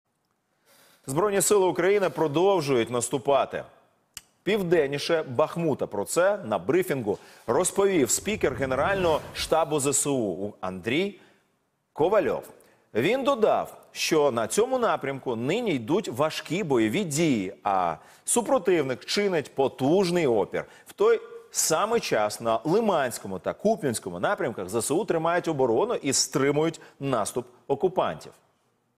Стали відомі деталі наступу ЗСУ біля Бахмута
Українські воїни контрнаступають біля міста Бахмут Донецької області.